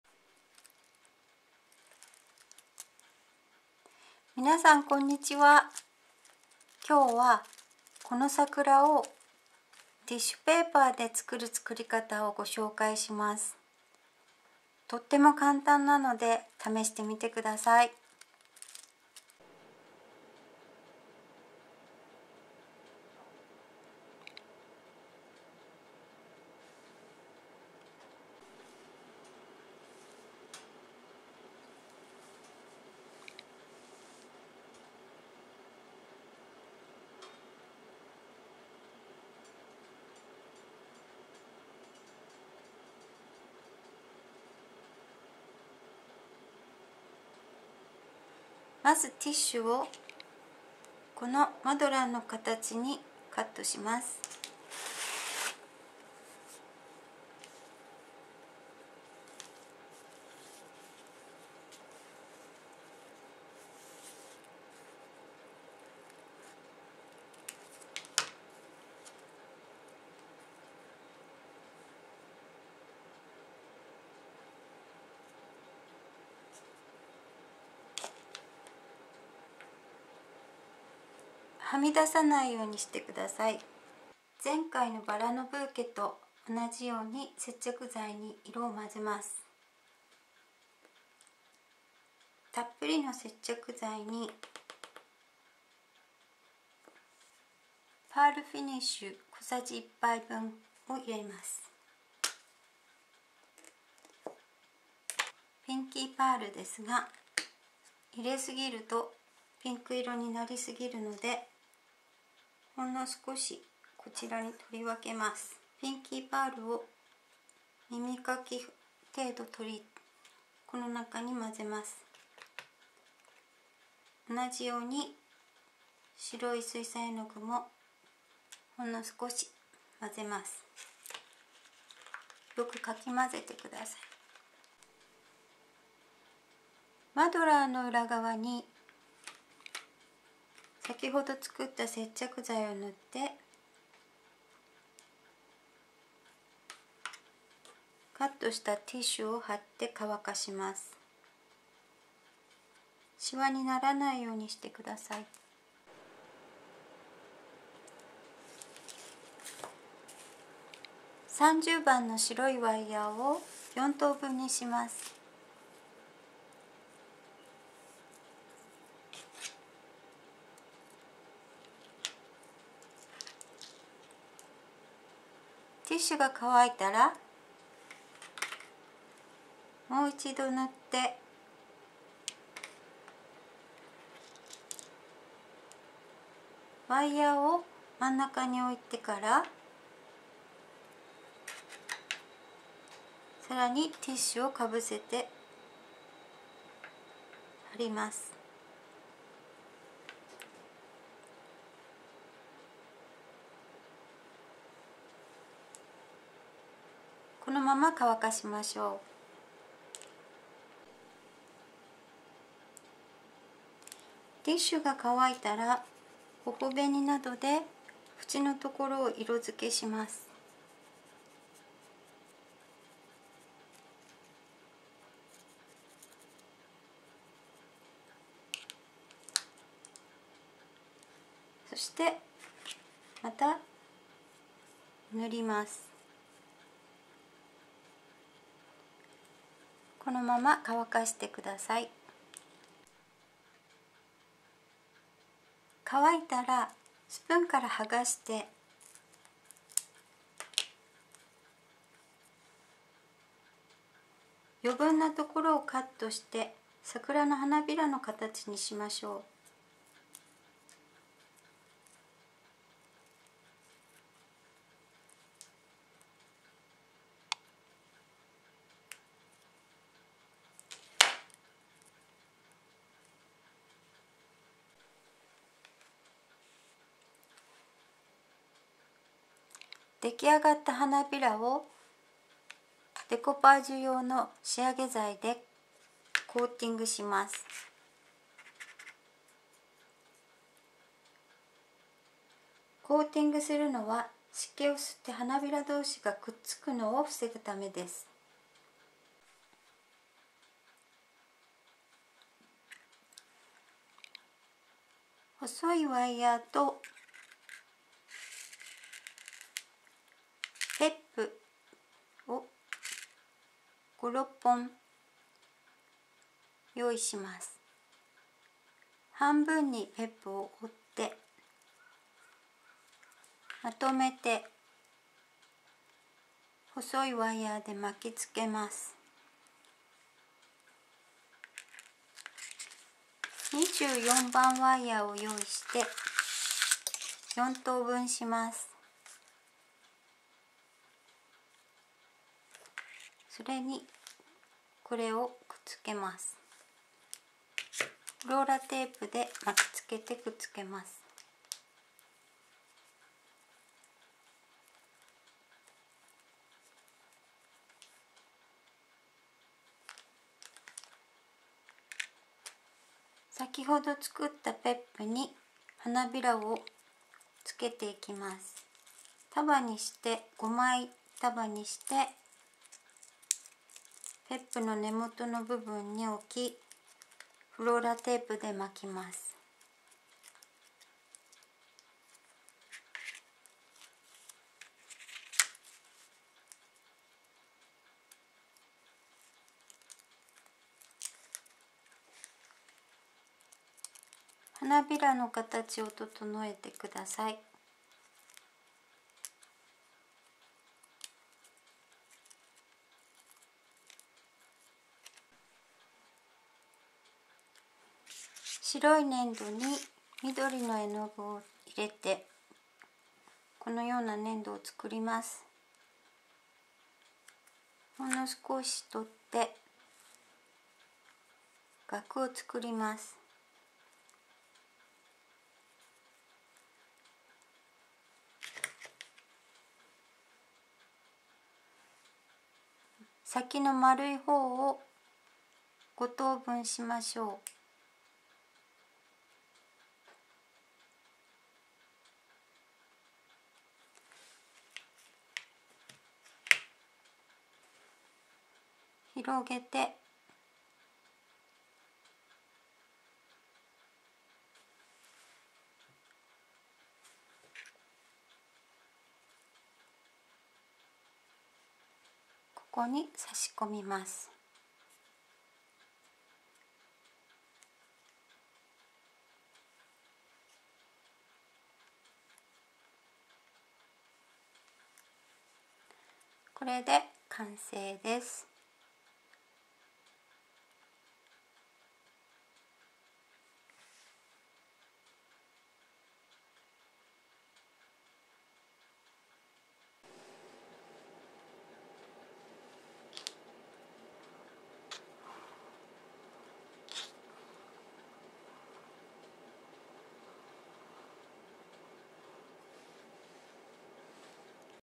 How to make Cherry Blossom from Tissue with original method
Easy and original method of making Cherry Blossom with regular tissue. So try it !Cherry Blossom is one of my favorite blossom.
私の好きな花の一つで，桜の花を簡単にティシペーパーで作れる方法をご紹介します！試してみてね！